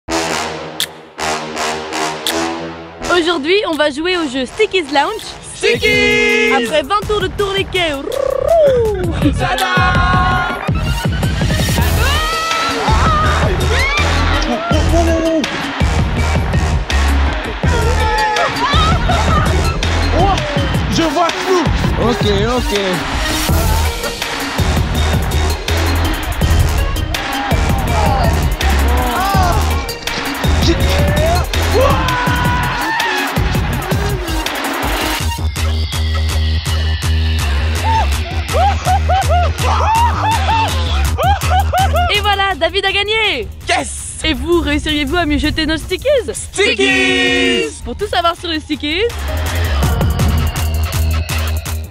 Stikeez Launch
Jeter un Stikeez c’est bien, coller un Stikeez c’est mieux. Choisis une cible et un Stikeez. Le premier qui met dans le mille a gagné !